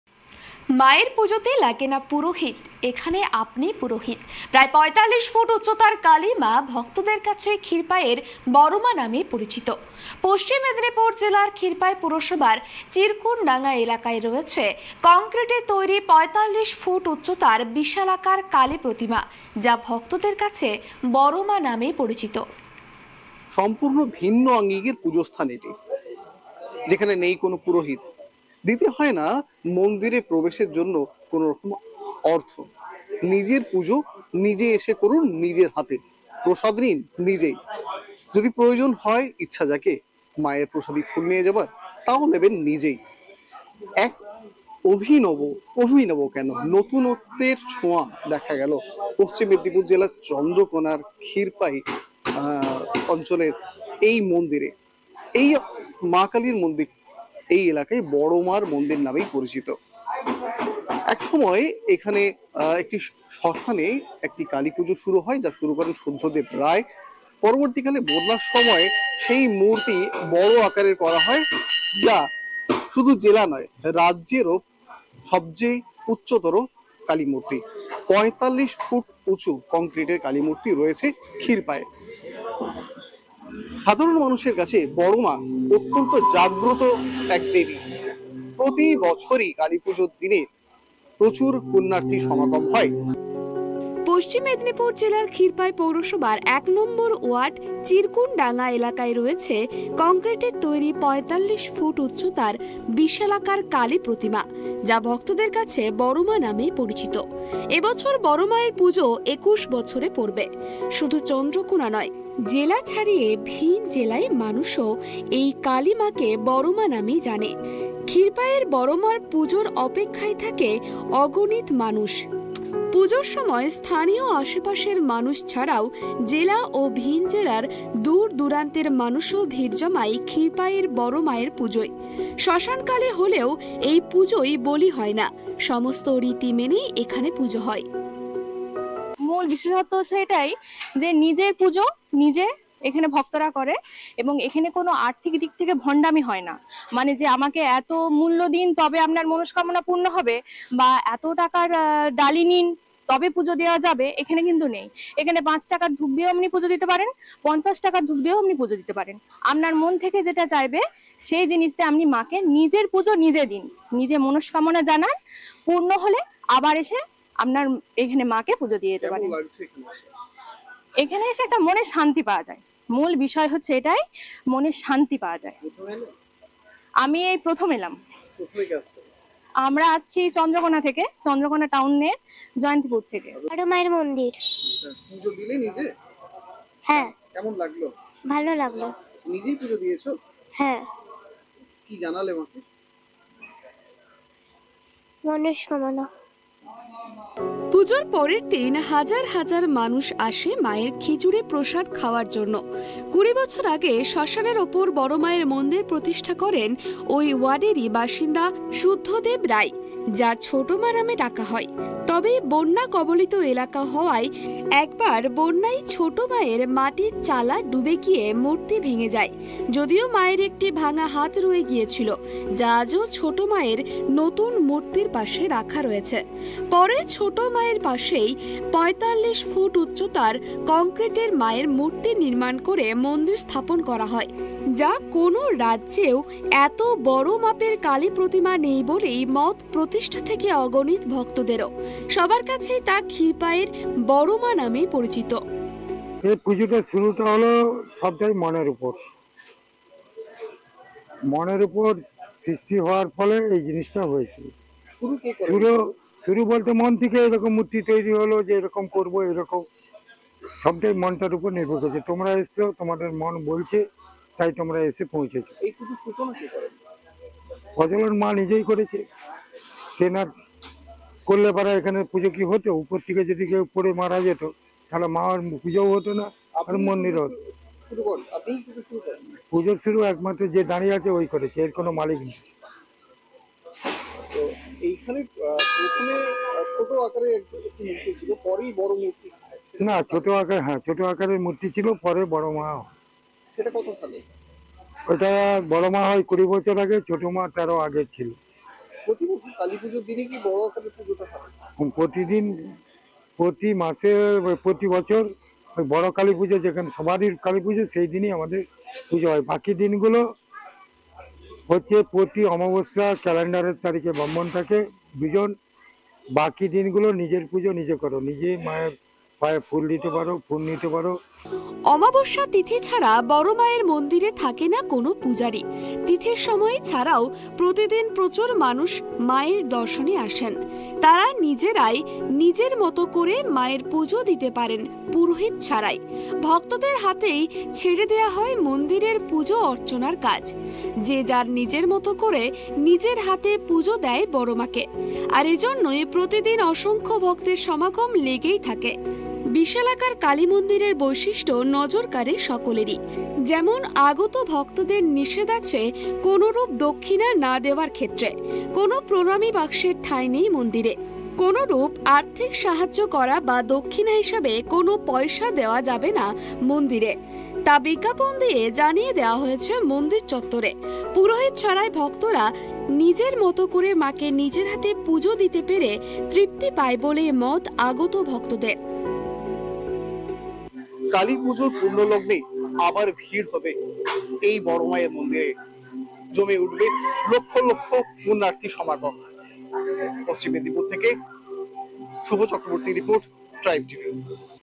পুরোহিত ছাড়াই হয় কালীপুজো, ‘বড়মা’র টানে ছুটে আসেন দূরদূরান্তের ভক্তরা
Tribe TV is now available on :
MEGHBELA- 277
DIGI-CABLE- 949
MRMPL- 764
BHORER ALO- 813
SGCN- 79
CGSN- 319